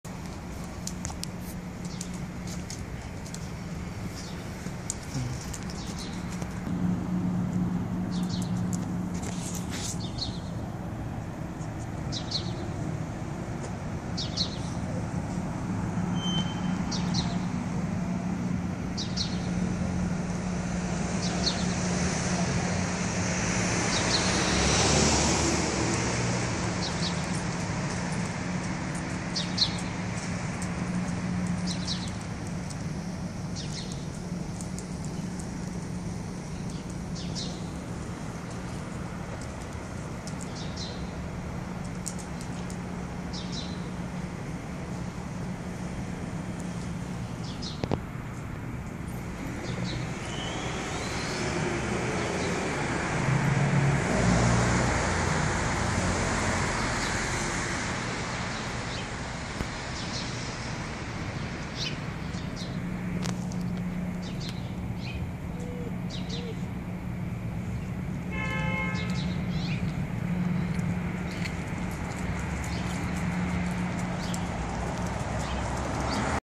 V8 Supercar Cloud C6 Corvette Cabrio
Proportions are quite spot on for the C6 Grand Sport Convertible. I've recorded a rainbow, a blueish optical atmospheric anomaly all while doing the video.